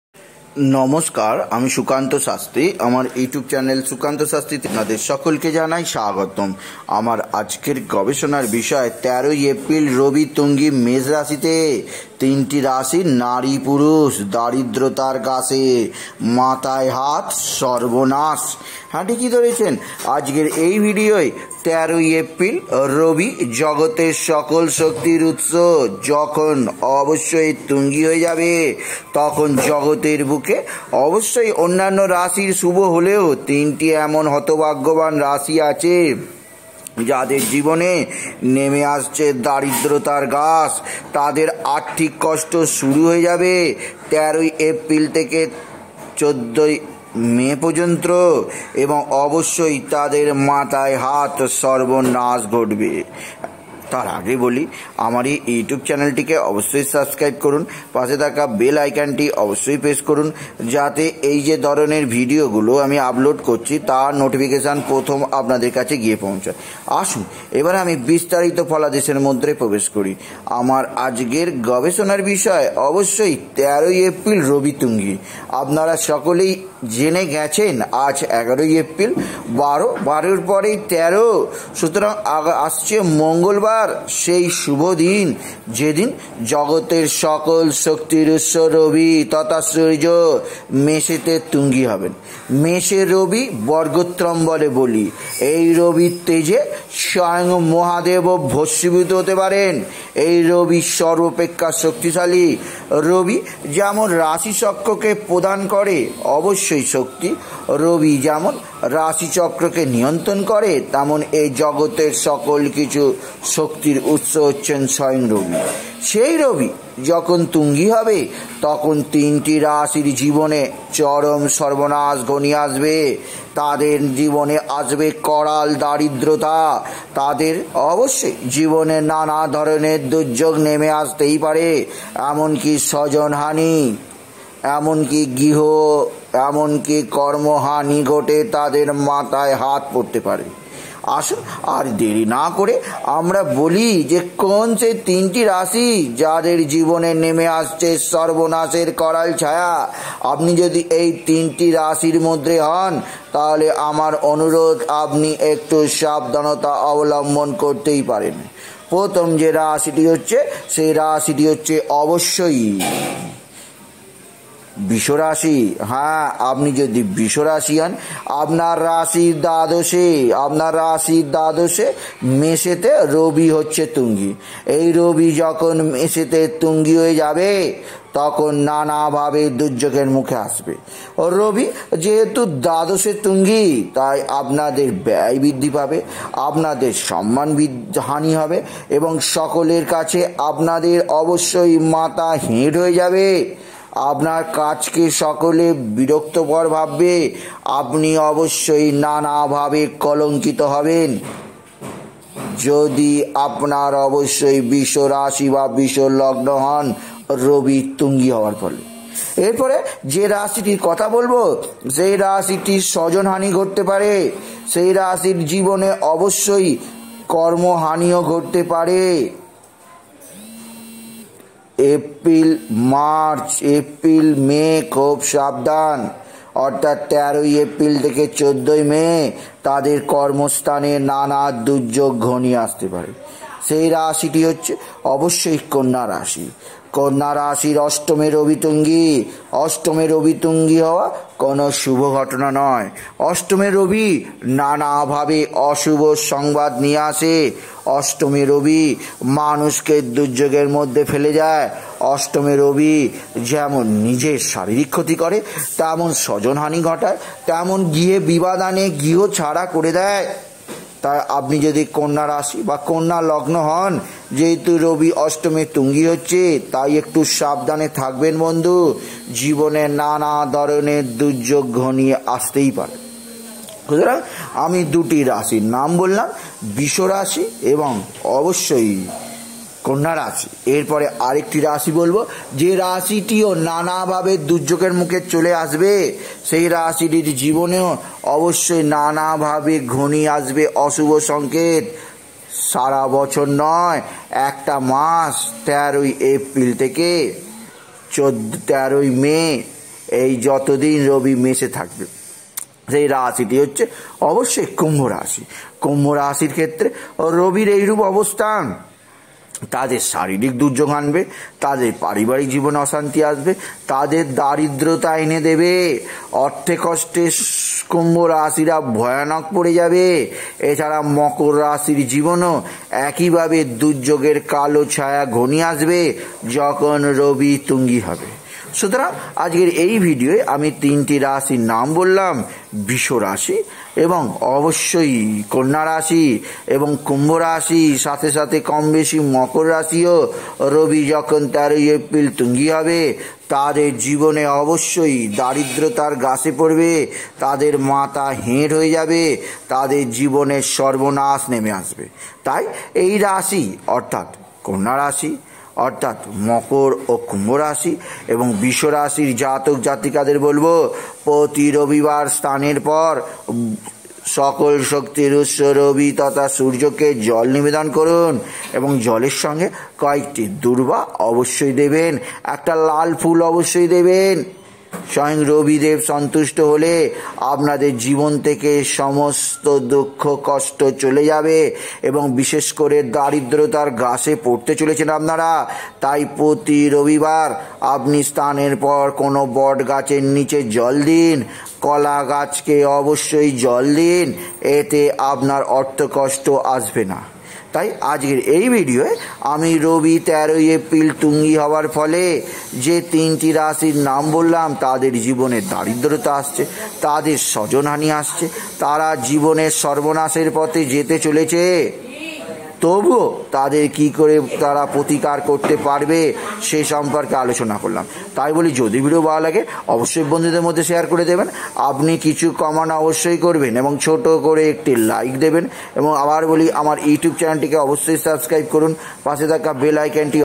১৩ই এপ্রিল রবি তুঙ্গী মেষ রাশিতে তিনটি রাশির নারী পুরুষ দারিদ্রতার গ্রাসে, সর্বনাশ, মাথায় হাত।
রবি তুঙ্গী হবার ফলে তিনটি রাশির অশুভ সময় শুরু হবে।